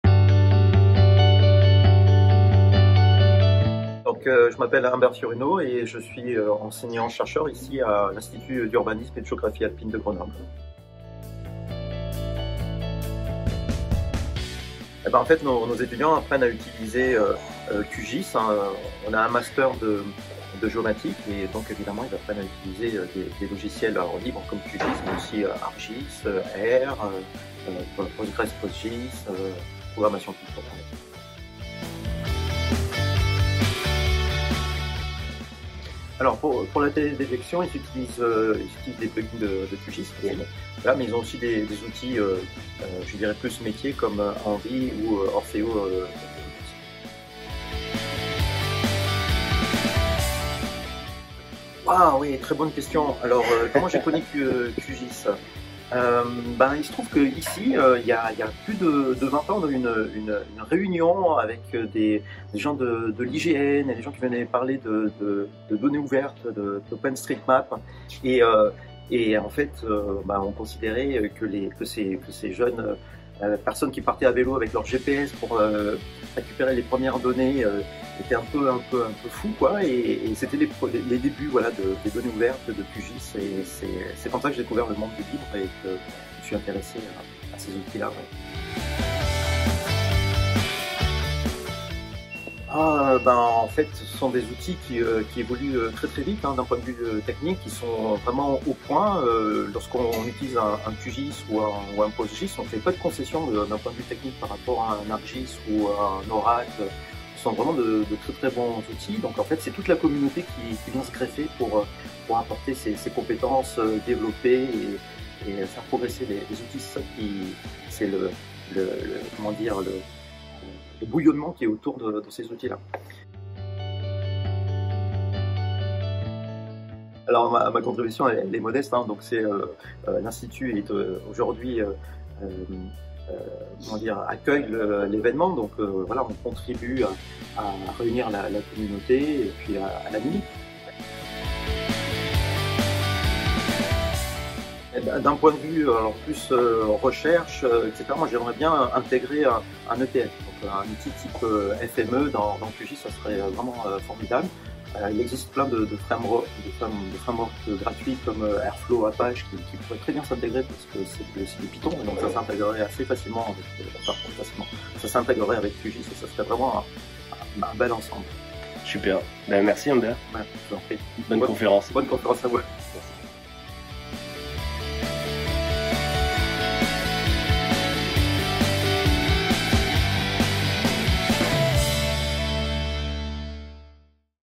Journées QGIS FR 2024 - GeoITW de Humbert Fiorino
Mini interview réalisée pendant les rencontres des utilisateurs francophones de QGIS ( @qgisfr8230  ) organisée les 27 et 28 mars 2024 à Grenoble par l'OSGeo-FR ( @osgeo-fr7212 ) et l'Université Grenoble Alpes ( @UniversiteDeGrenoble ).

Questions :

- Ces journées sont un moment fort de l'association OSGeo-FR ?
- Un moment que tu as particulièrement apprécié durant l'événement ?
- Comment se prépare un tel événement ?
- Qu'est-ce que tu retiens de la conférence ?